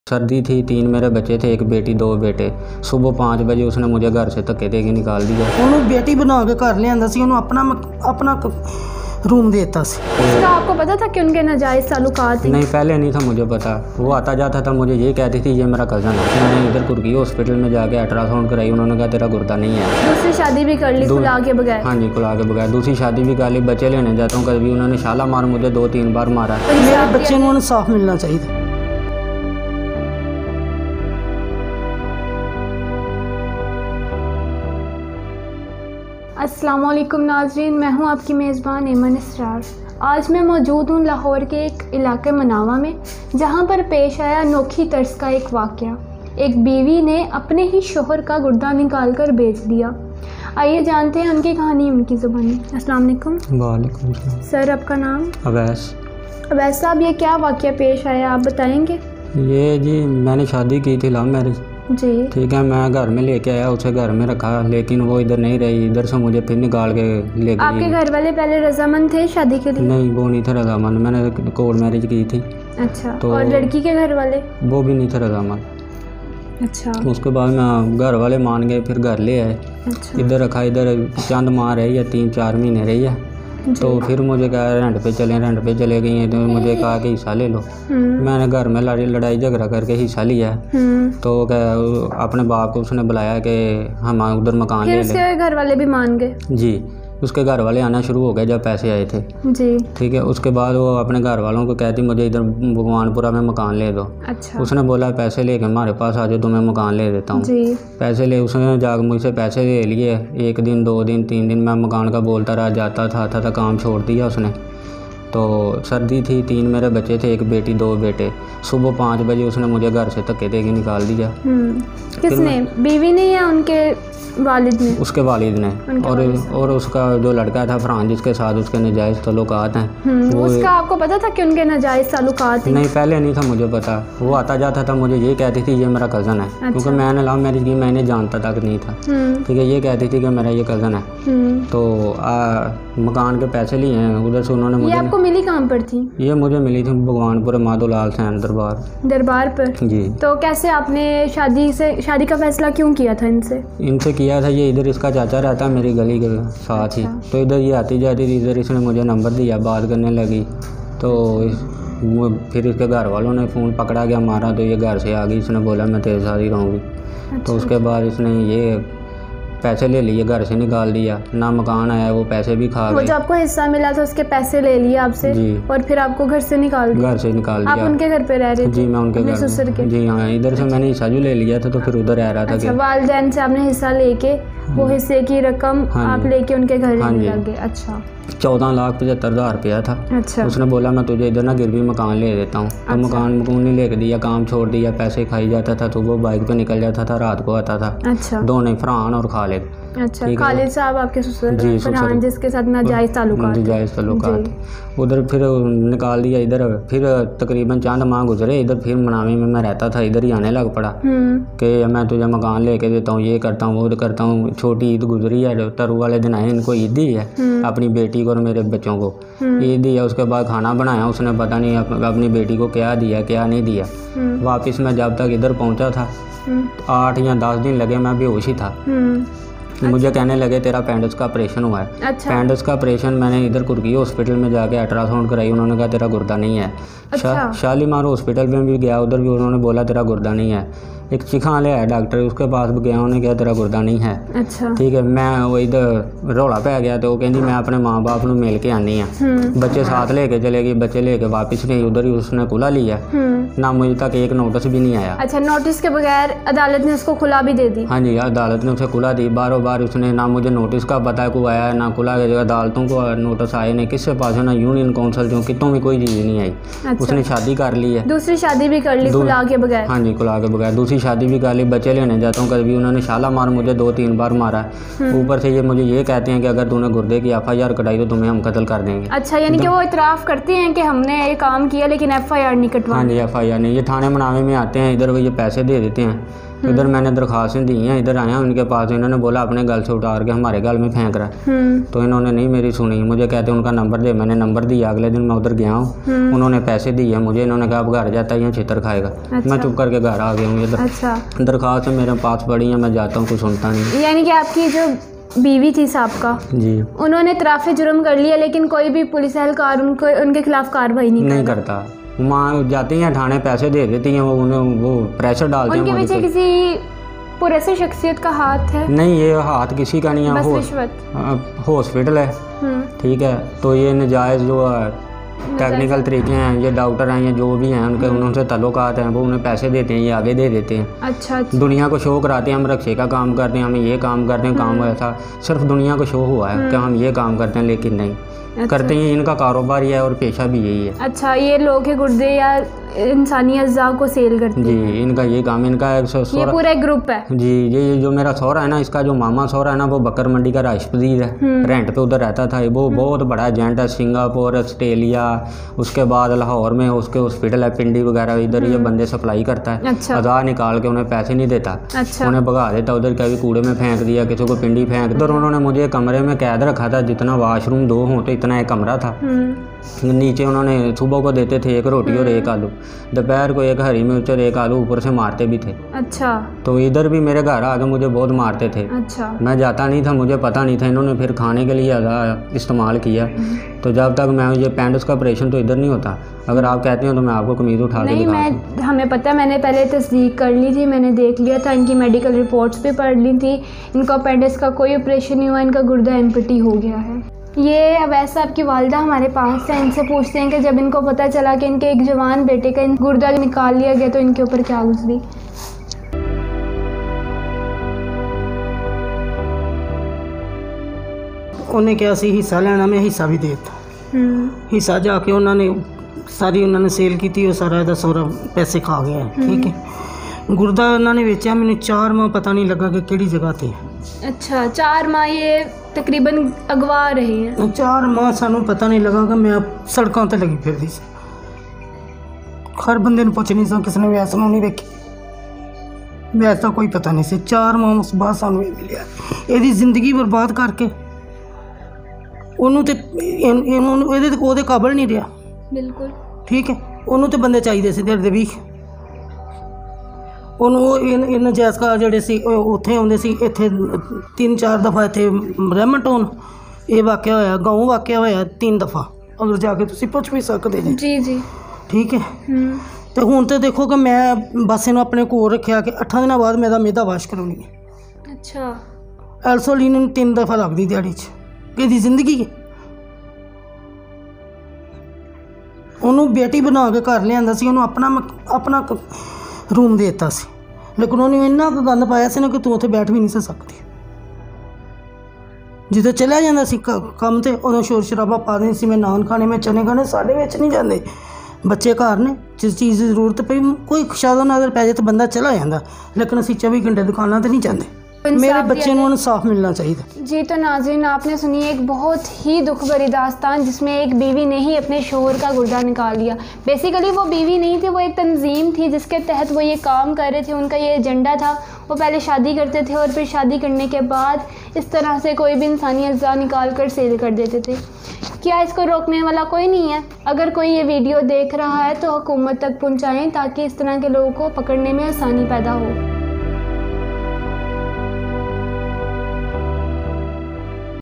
Bewafa Bivi Ney Ashiq Key Sath Mil Kar Shohar Ka Gurda Hi Baich Diya | Sad Story of Husband Wife
Bewafa Bivi Ney Ashiq Key Sath Mil Kar Shohar Ka Gurda Hi Baich Diya | Sad Story of Husbund Wife